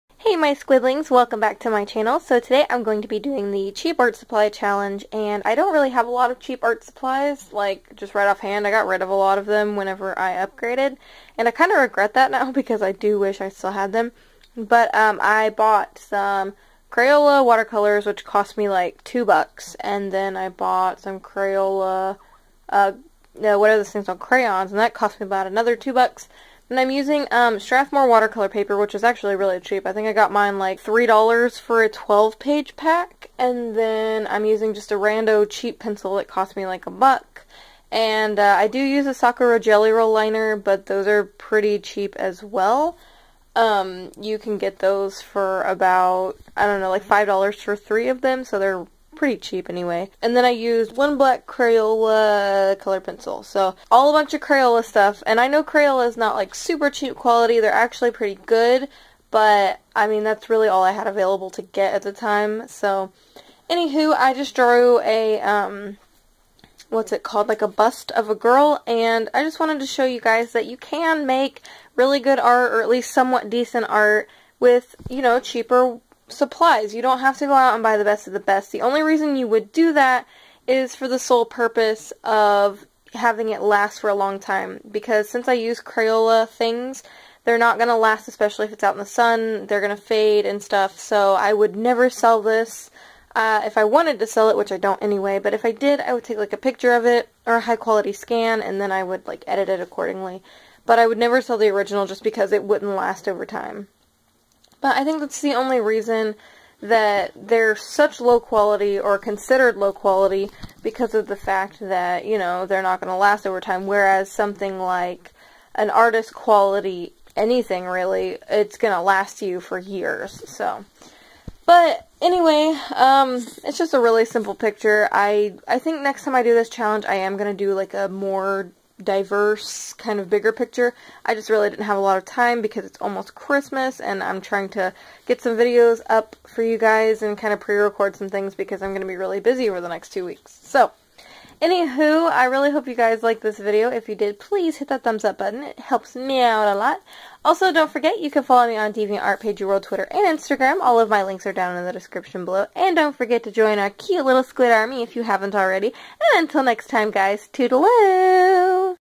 CHEAP ART SUPPLY CHALLENGE
Hey my Squidlings! Today I am (attempting) to do the cheap art supply challenge! I used Crayola, since they were more easily available to me! This just goes to show that you don't necessarily need to have the latest and greatest supplies to create!

Final finished piece: -

Materials used:
Crayola Watercolors
Crayola Crayons
Crayola Black Colored Pencil
Strathmore Cold Press Watercolor Paper
Field Notes HB Pencil
Sakura Gelly Roll Pen White

Time taken:
16 mintues